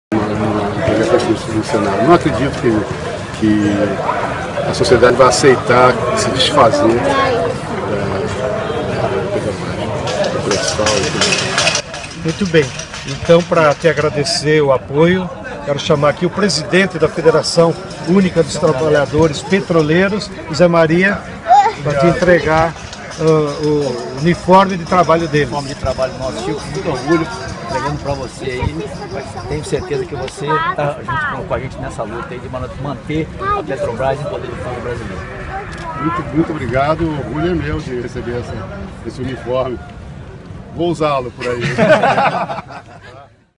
Chico Buarque em defesa da Petrobras (Politheama X Movimentos Sociais)
Em jogo de futebol entre o o time de Chico Buarque e o time dos movimentos sociais, Chico Buarque declara seu apoio à Petrobras e ao Pré-Sal. Zé Maria Rangel, presidente da Federação Única dos Petroleiros, presenteia o compositor com o uniforme da FUP. Não Perca!